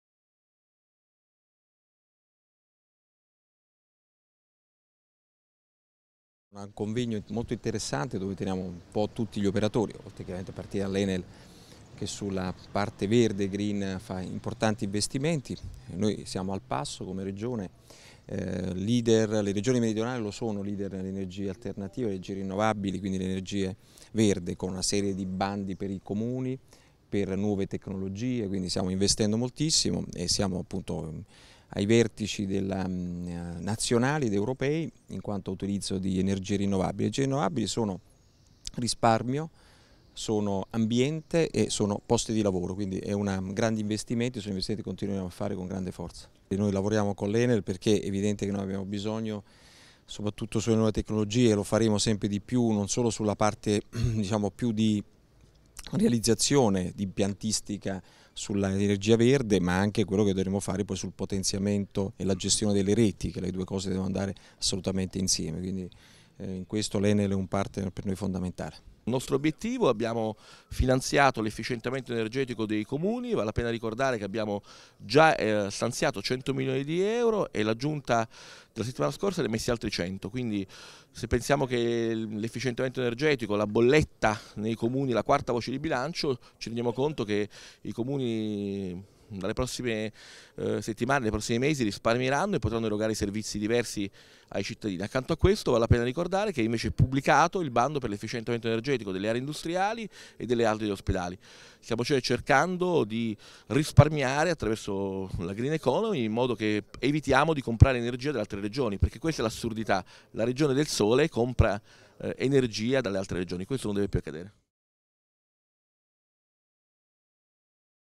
Efficienza energetica per una nuova crescita
"Efficienza energetica per una nuova crescita. Le sfide per un'energia accessibile, sicura e sostenibile" è il tema del convegno promosso dalla Regione Campania e dall' Enel tenutosi presso la Sala Ischia della Mostra d'Oltremare. I lavori sono stati aperti con gli interventi dell'assessore alle Attività produttive Fulvio Martusciello e del rettore dell'Università del Sannio Filippo de Rossi. a seguire la presentazione da parte di Vittorio Chiesa, professore ordinario del Politecnico di Milano, del rapporto realizzato dall'Università e da Enel Foundation su "Stato e prospettive dell'efficienza energetica in Italia"; l'intervento di Mauro Libè, consigliere politico del ministro dell'Ambiente e della Tutela del Territorio e del Mare, su "Le linee del governo sull'efficienza energetica"; infine una tavola rotonda su "La cultura dell'efficienza" a cui hanno preso parte il commissario Enea Giovanni Lelli, il direttore della Divisione Generazione dell'Enel Gianfilippo Mancini, il presidente di Assoelettrica Chicco Testa e il vicepresidente di Legambiente Edoardo Zanchini. Il dibattito è stato moderato dal caporedattore centrale del TGR RAI Campania Antonello Perillo. A concludere i lavori il presidente della Regione Campania Stefano Caldoro.

La manifestazione è stata l'occasione per presentare i bandi della Regione Campania, co-finanziati dal Fesr per l'efficientamento energetico dei comuni.